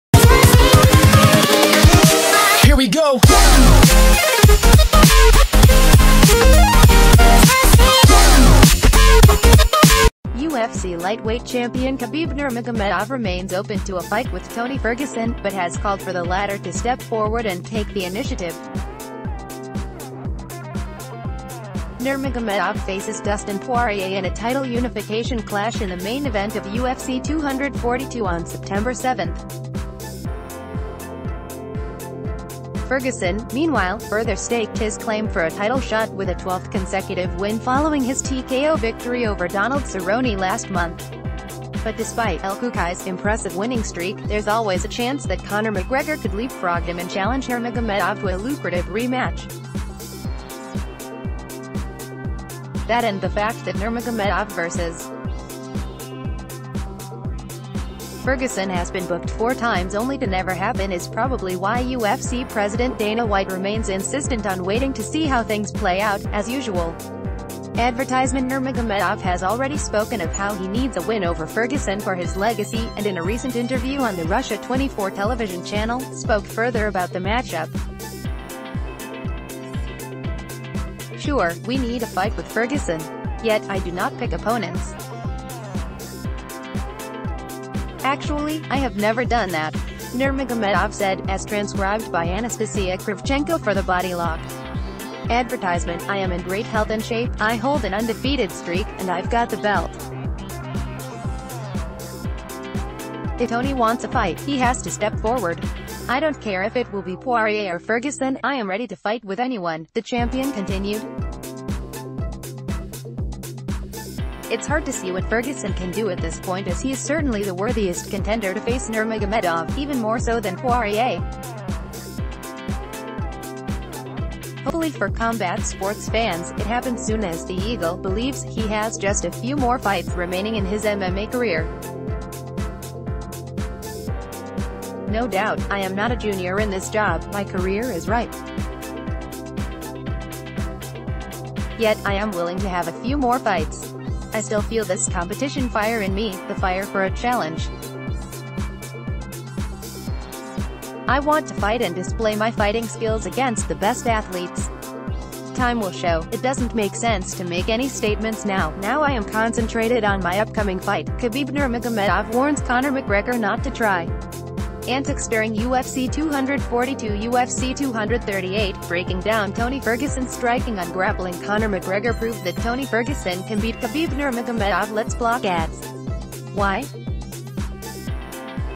Khabib Nurmagomedov calls for Tony Ferguson to 'step forward' if he wants a title fight
✔ UFC lightweight champion Khabib Nurmagomedov remains open to a fight with Tony Ferguson, but has called for the latter to step forward and take the initiative. Nurmagomedov faces Dustin Poirier in a title unification clash in the main event of UFC 242 on September 7. Ferguson, meanwhile, further staked his claim for a title shot with a 12th consecutive win following his TKO victory over Donald Cerrone last month. But despite “El Cucuy’s” impressi...

✔ You can see the details of "Khabib Nurmagomedov calls for Tony Ferguson to 'step forward' if he wants a title fight| MMA UFC" here